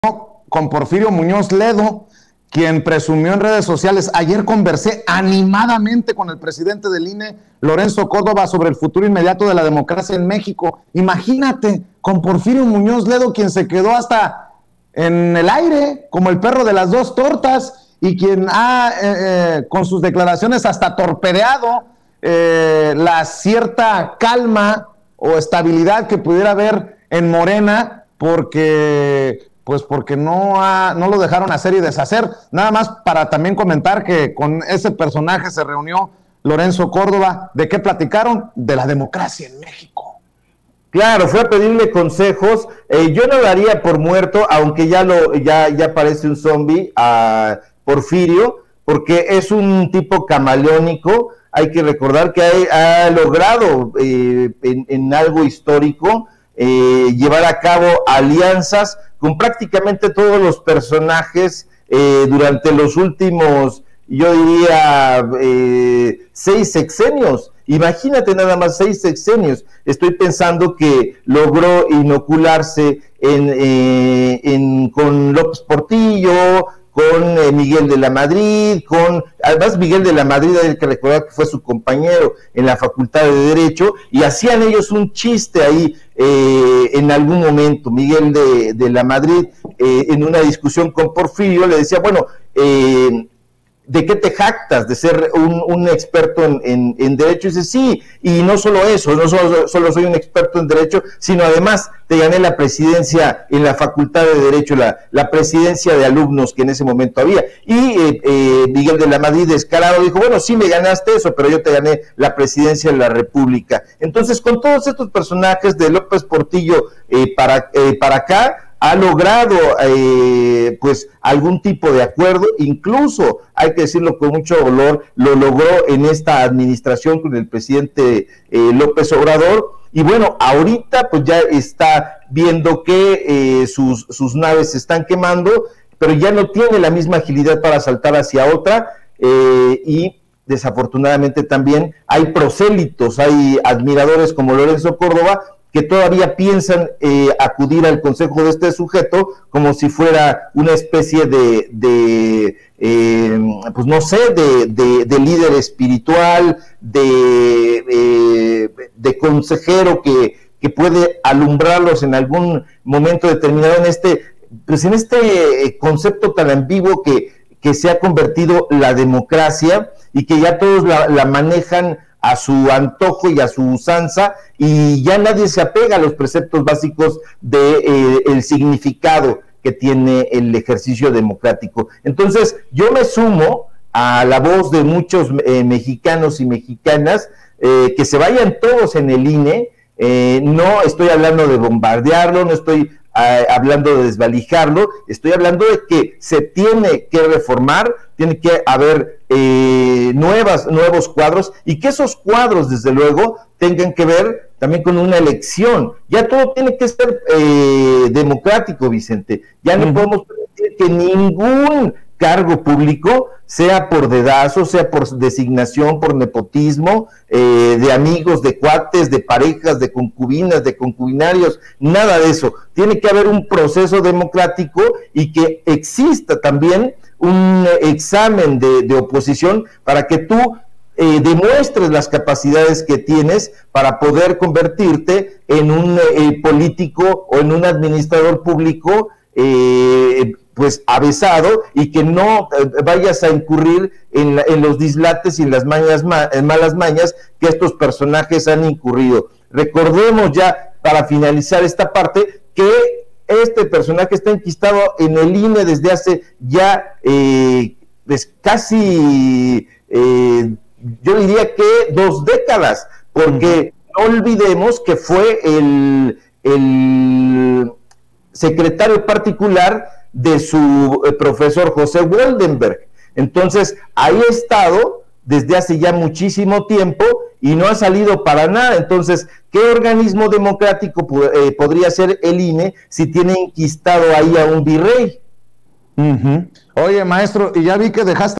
RICARDOSEVILLA DICE QUE MUÑOZ LEDO YA NO TIENE LA MISMA AGILIDAD PARA SALTAR DE UN LADO A OTRO
Lorenzo Córdova se reunió recientemente con Porfirio Muñoz Ledo, quien se quedó hasta en el aire como el perro de las dos tortas, pero que bien que torpedeó la calma en Morena porque no lo dejaron hacer y deshacer. El escritor y periodista colaborador de RegeneraciónMX y Polemón RicardoSevilla nos comenta que él daría por muerto a Porfirio a pesar de los camaleónico que resultó con tantos otros personajes políticos desde López Portillo; dice que ya no tiene la misma agilidad para saltar de un lado a otro